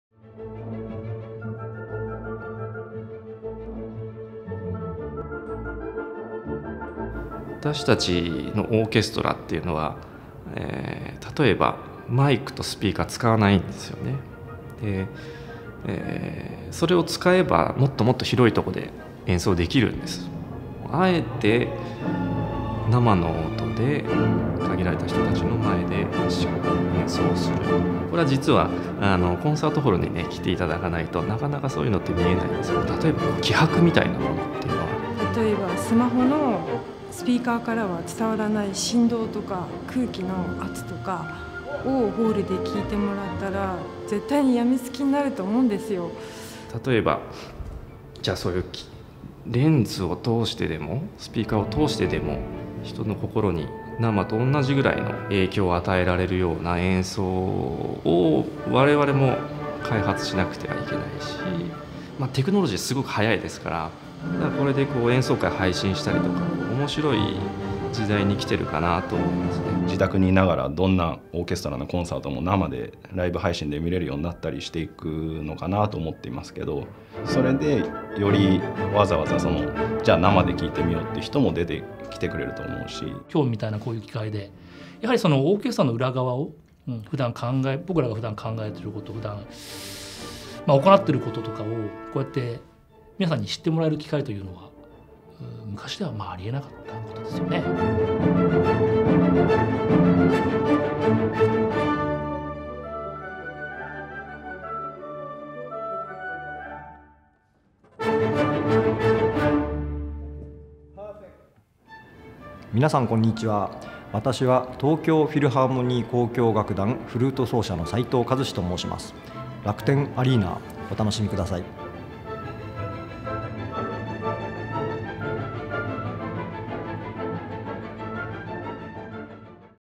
The Future of Classical Music - The Tokyo Philharmonic Orchestra
"Technology develops so rapidly. Now we can stream live music and it's an interesting time to be a performer" 🎼

Find out what the future holds for classical music and the Tokyo Philharmonic Orchestra 🎻